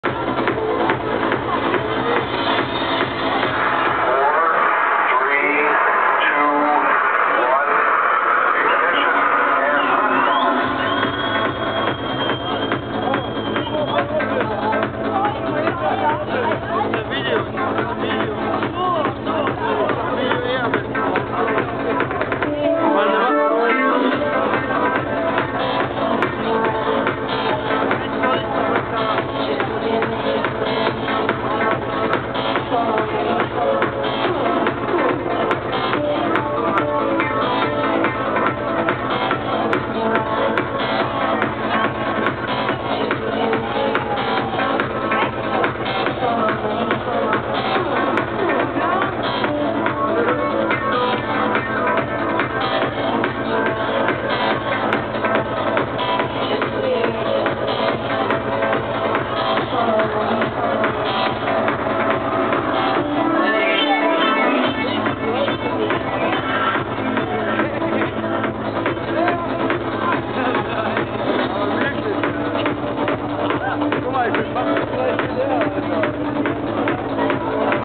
*****LUNA CLUB***** NEW YEARS EVE 2009 !!!PLANET IS BACK!!! 1
new years eve @ planet bochum (the planet with 2 moons)

...i think